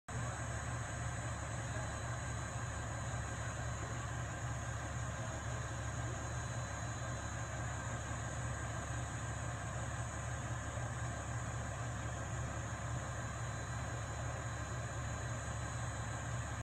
Beach Haven Fire Company Engines 1501 and 1511 On Scene
On scene of an automatic fire alarm. 08/06/17.